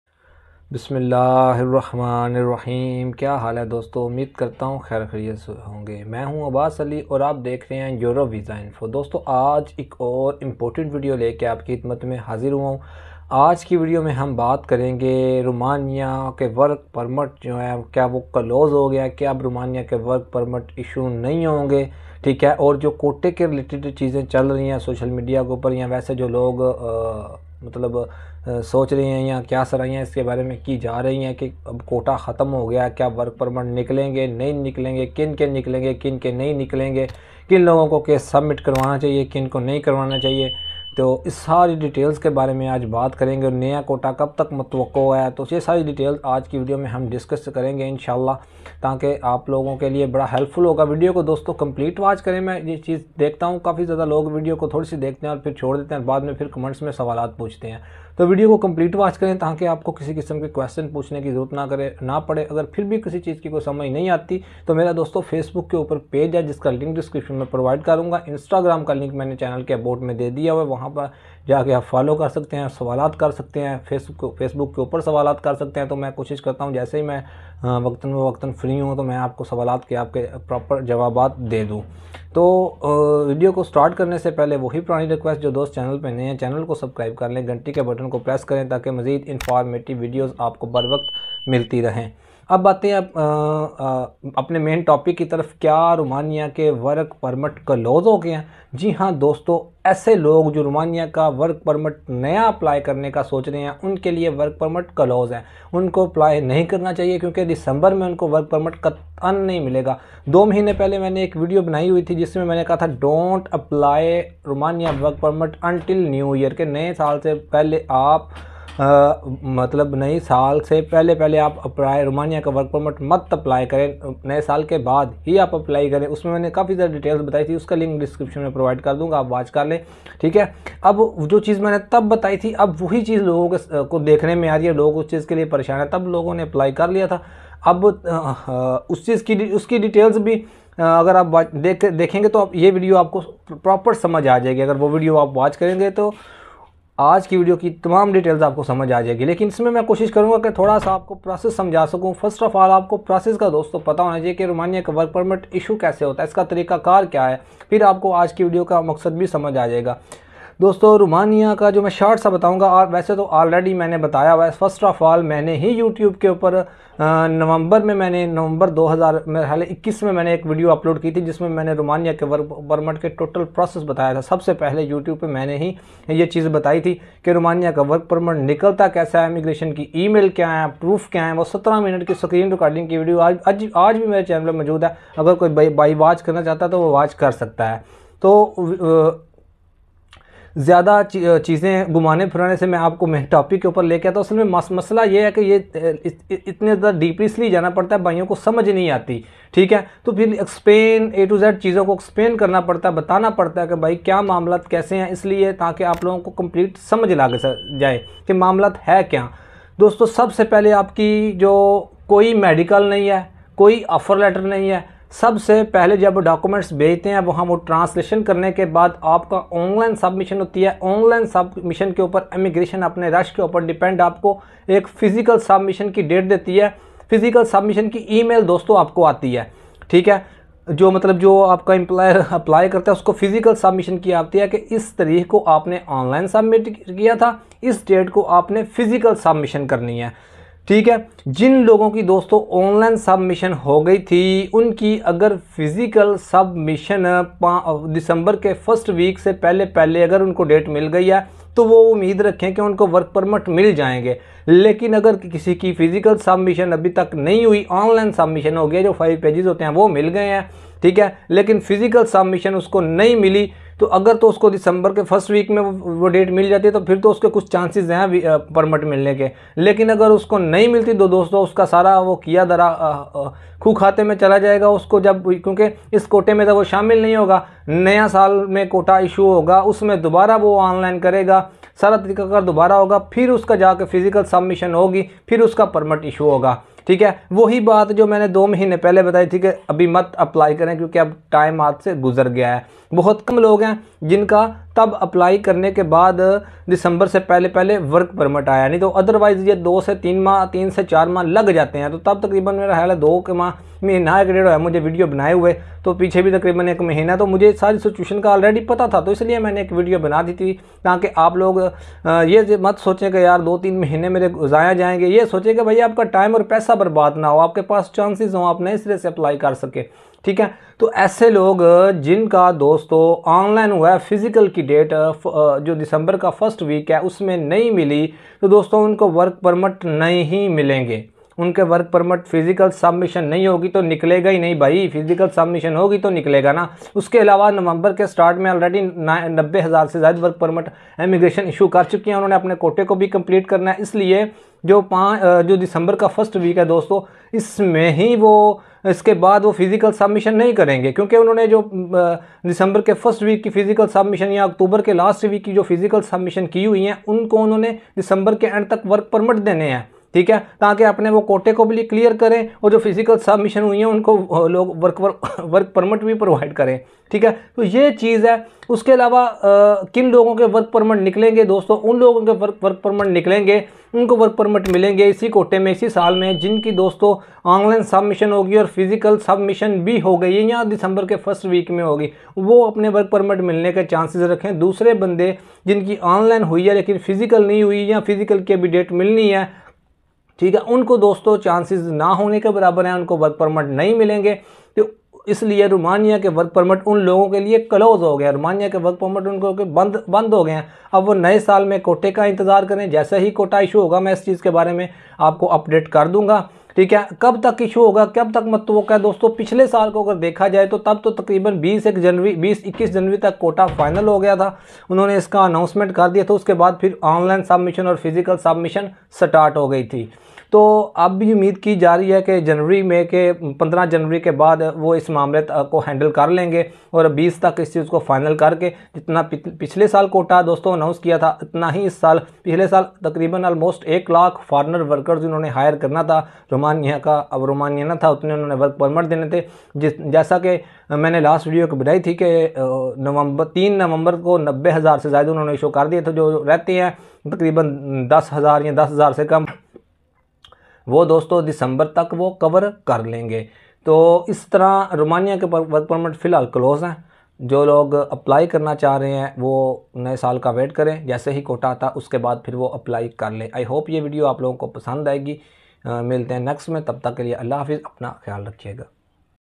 Romaina 🇷🇴 work permit closed now 🔒| Romaina work permit update | Romaina visa update
Facebook page ko like karin or koi bat pochni ho tu Facebook pay mesg Karin

Aj ki video main hum bat karin k Romaina imgration nay is sal kitny work permit issues kiye or next year's kitny karny ki ummed ha

 Romania work permit process video link here

 Romania embessy appointment confirmation k proofs

Romania embessy main interview main kon say questions pochy jaty Hain